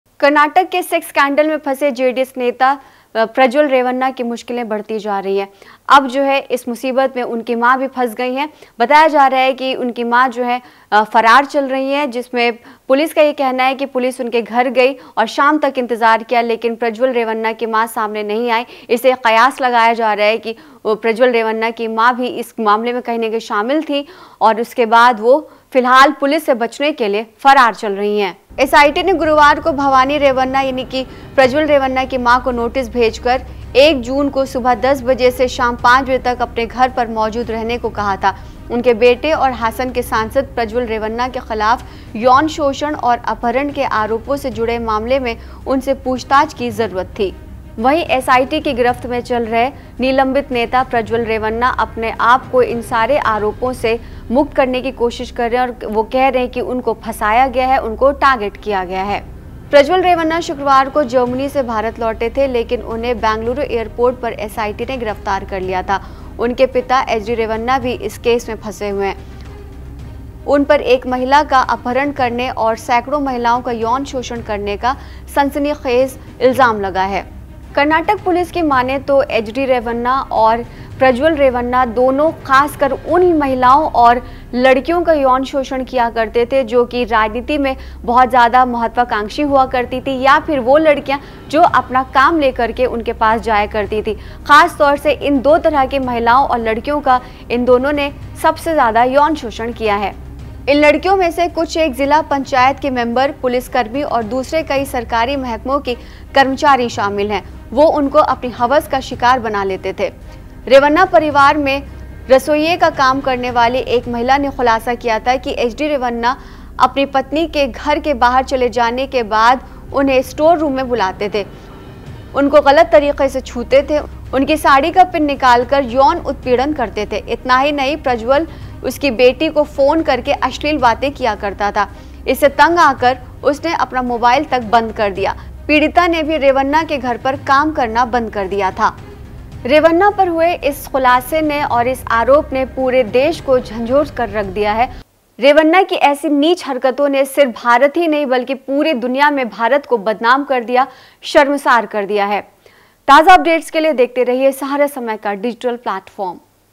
Karnataka Scandal: प्रज्वल रेवन्ना की मां फरार
कर्नाटक के चर्चित सेक्स स्कैंडल में फंसे जेडीएस नेता प्रज्वल रेवन्ना की मां भवानी रेवन्ना अपने घर से फरार हैं. एसआईटी उनसे पूछताछ के लिए हासन जिले के होलेनरसिपुरा में स्थित उनके घर पहुंची थी, लेकिन वो पहले ही घर जा चुकी थी. पुलिस ने शाम तक उनका इंतजार करती रहीhindinews